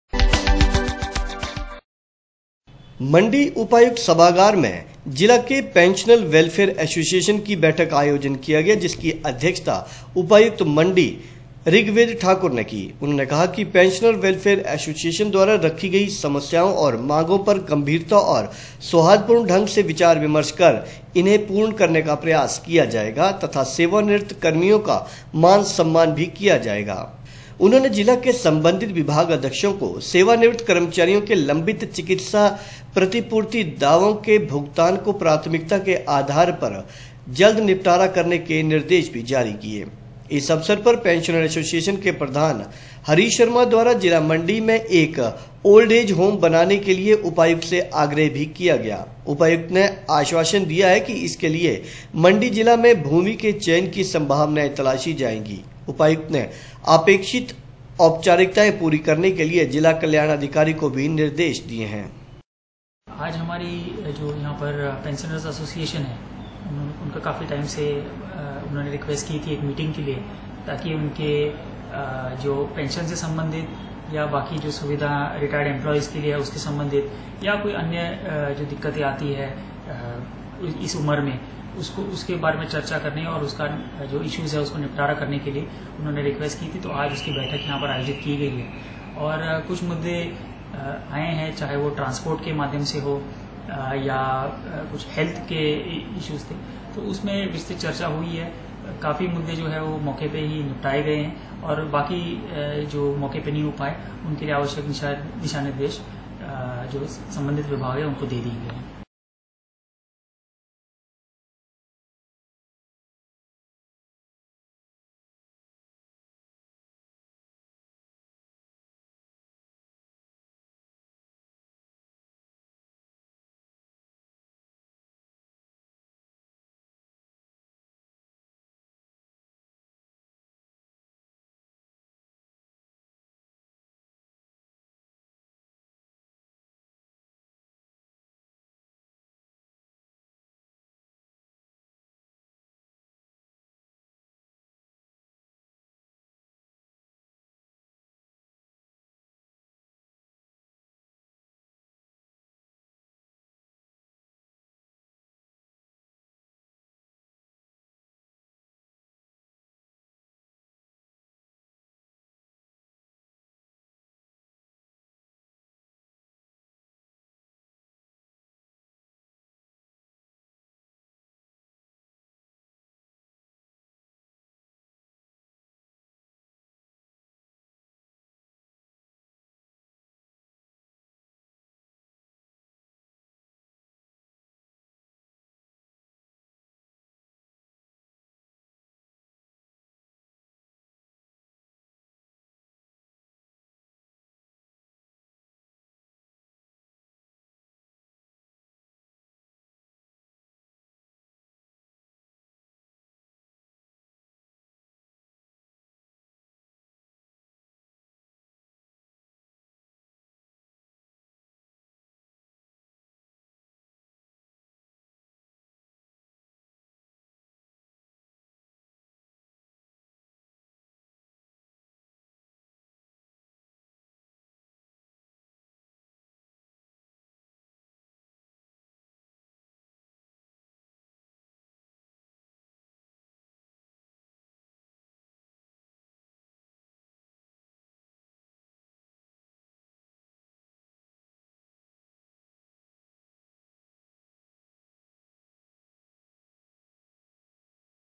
उपायुक्त सभागार मण्डी में आज जिला के पैंशनर वैलफेयर एसोसियेषन की एक बैठक
उपायुक्त सभागार मण्डी में आज जिला के पैंशनर वैलफेयर एसोसियेषन की एक बैठक का आयोजन किया गया, जिसकी अध्यक्षता उपायुक्त मण्डी श्री ऋग्वेद ठाकुर ने की।  उन्होंने कहा कि पैन्षनर वैलफेयर एसोसियेशन द्वारा रखी गई समस्याओं और मांगों पर गम्भीरता व सौहार्दपूर्ण ढंग से विचार-विमर्श कर इन्हें पूर्ण करने का प्रयास किया जाएगा तथा सेवा-निवृत कर्मियों का मान-सम्मान भी किया जाएगा।  उन्होंने बताया कि पैंशनर गृह के निर्माण के लिए भूमि की स्वीकृति सरकार से प्राप्त हो गई है तथा अपेक्षित औपचारिकताएं पूरी की जा रही हैं।